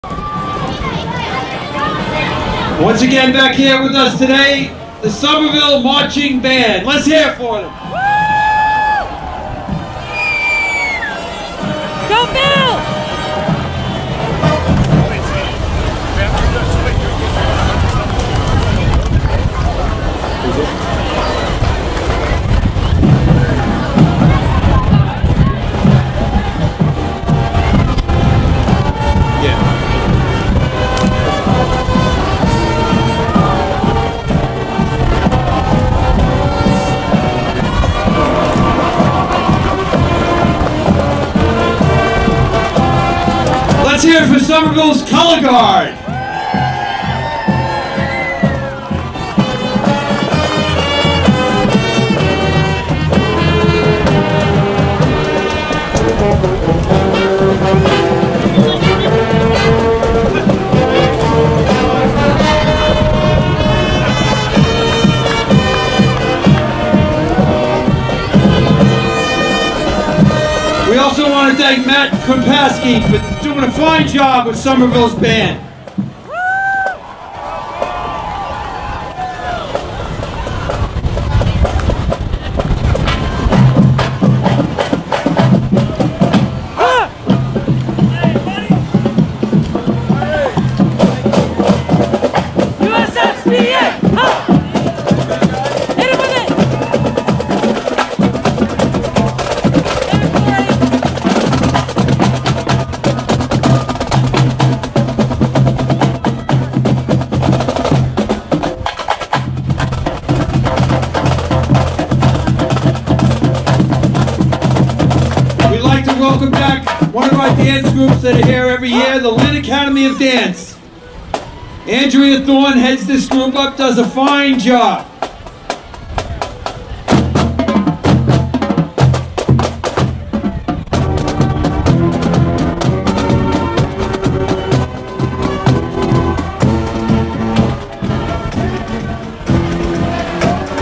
SHSMB St Patrick's Day 22 4K
Somerville High School performs at the Somerville, NJ St. Patrick's Day Parade, March 2022.